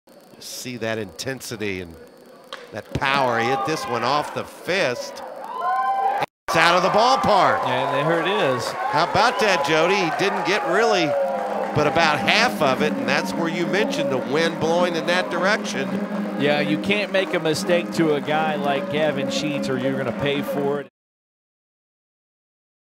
Wake Forest's Gavin Sheets Muscles Out 20th HR Of Season
Wake Forest infielder Gavin Sheets muscled his 20th home run of the season out of the ballpark in the 9th inning against Georgia Tech Thursday. The bomb started a four-run rally that put the Demon Deacons on top for good in their ACC Championship win over the Yellow Jackets. Sheets is the first Wake Forest player since 2003 to hit at least 20 homers in a season. Check out how Sheets went deep despite appearing to get jammed in this ACC Must See Moment!